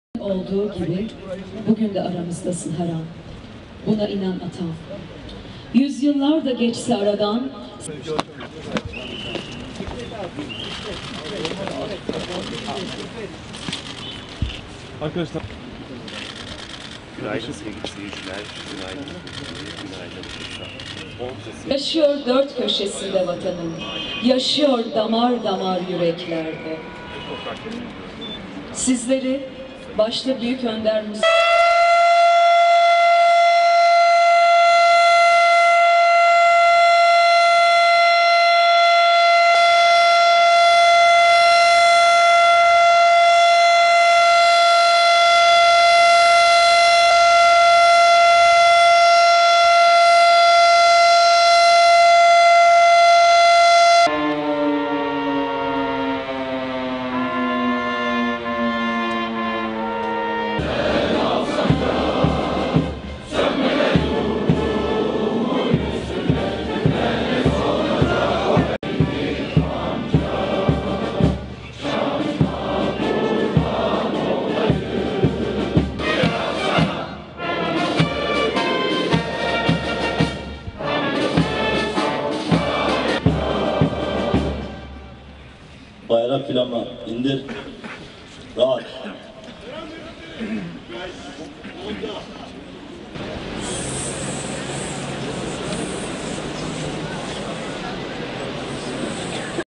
Bursa'da hayat durdu!
Türkiye Cumhuriyeti'nin kurucusu Mustafa Kemal Atatürk, ebediyete intikalinin 79'uncu yılında Bursa'da anıldı. Atatürk'ün hayata gözlerini yumduğu saat 09:05'te sirenler çaldı, vatandaşlar caddelerde saygı duruşunda bulundu.